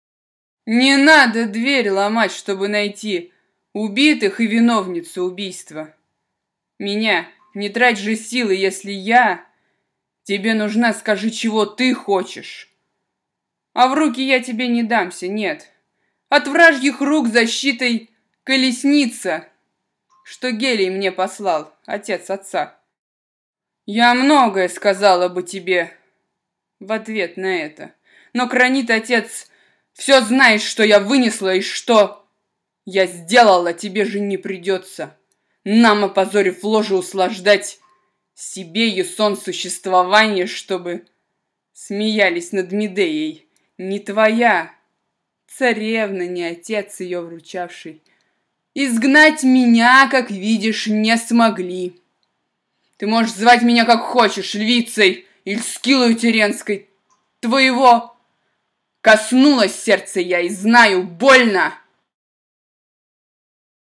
Medea Monologue Evripid
Монолог Медеи читает Мария Соловьева
Medea Monologue (Euripides) performed by Maria Solovyova

                     Не надо дверь ломать, чтобы найти
                     Убитых и виновницу убийства -
                     Меня. Не трать же сил и, если я
                     Тебе нужна, скажи, чего ты хочешь.
                     А в руки я тебе не дамся, нет:
                     От вражьих рук защитой - колесница,
                     Что Гелий мне послал, отец отца.
                     Я многое сказала бы тебе
                     В ответ на это. Но Кронид-отец
                     Все знает, что я вынесла и что
                     Я сделала. Тебе же не придется,
                     Нам опозорив ложе, услаждать
                     Себе, Ясон, существованье, чтобы
                     Смеялись над Медеей. Ни твоя
                     Царевна, ни отец, ее вручавший,
                     Изгнать меня, как видишь, не могли.
                     Ты можешь звать меня как хочешь: львицей
                     Иль Скиллою Тирренской; твоего
                     Коснулась сердца я и знаю - больно...
(Еврипид, перевод Иннокентия Анненского)